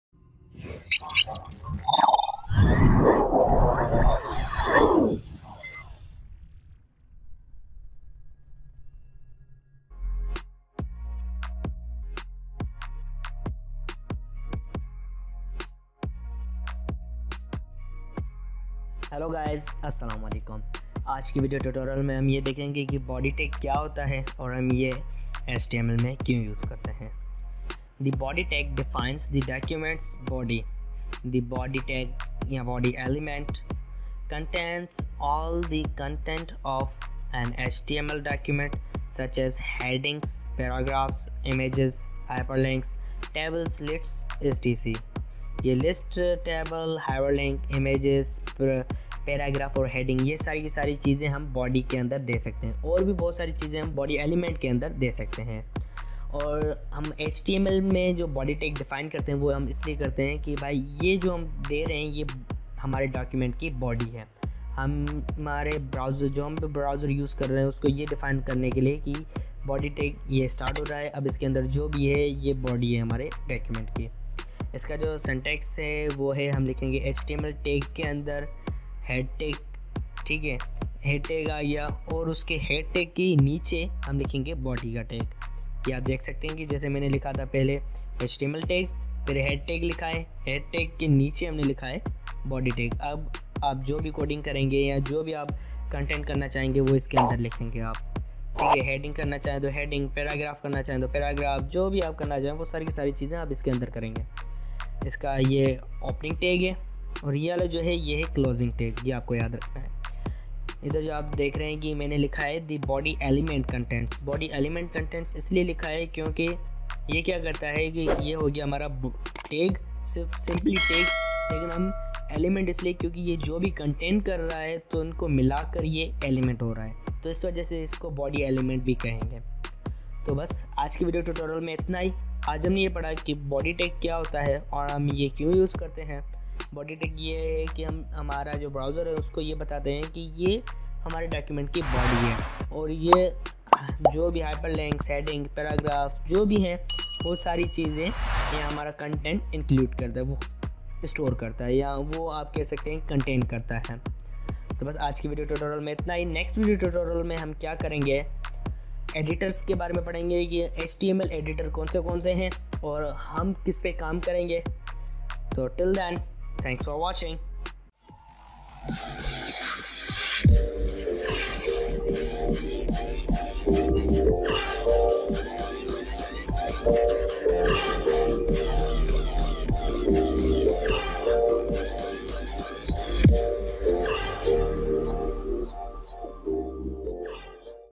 Use of BODY Tag in HTML | Hindi / Urdu | Programming Eye
It's essential for every Web Developer to understand the Use of BODY Tag in HTML. This tutorial is from Beginner Level to Advanced Level, you'll learn here how to be futures best Web Developer/Programmer.

Here's what you'll see 👀 in this video:
[Add timestamps of the sections in your video]

how to set top and left margin of webpage in hindi

Our channel is about Education.

Yasir Yousuf unnar
YouTuber/ CEO @ Programming Eye (a Software House)